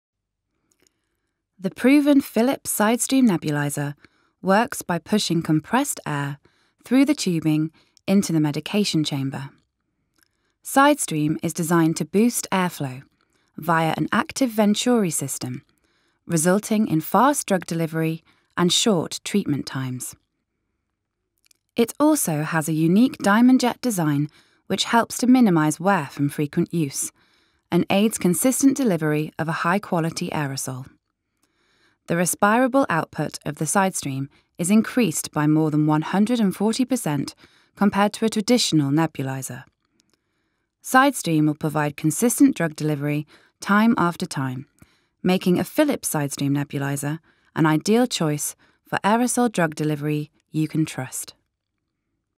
Technologie nebulizátoru SideStream | Philips
VIDEO JE URČENÉ PRO ODBORNÍKY

Pomáhá při čištění, zvlhčování a zmírnění ucpání dětských dýchacích cest.*
Technologie SideStream u inhalátorů Philips.

*při nebulizaci 0,9% fyziologického roztoku